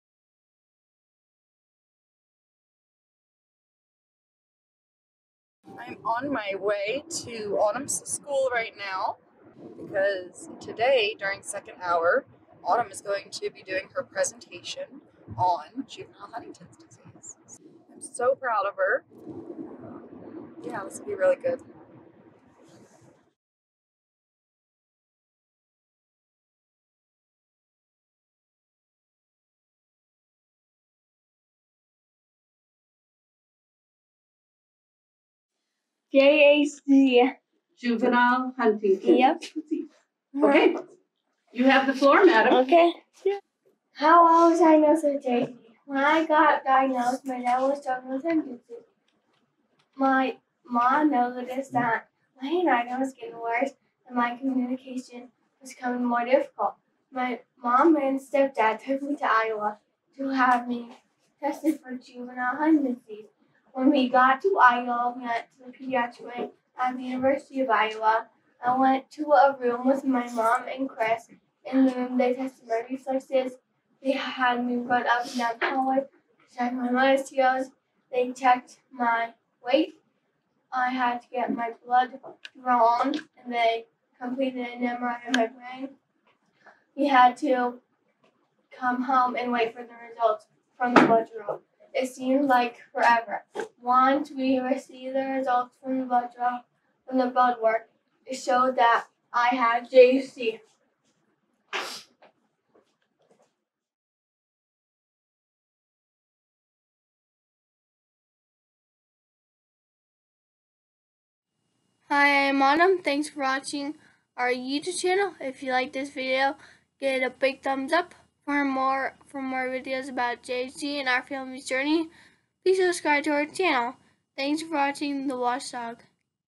A Lesson In JHD (preview) By Autumn
When Autumn was told that she needed to do a presentation on genetics,  she knew exactly what her topic would be.  Juvenile Huntions Disease.

Im so proud of her and I wanted to share this right away :) Please subscribe and click the notifications bell so that you will know when the full video is released.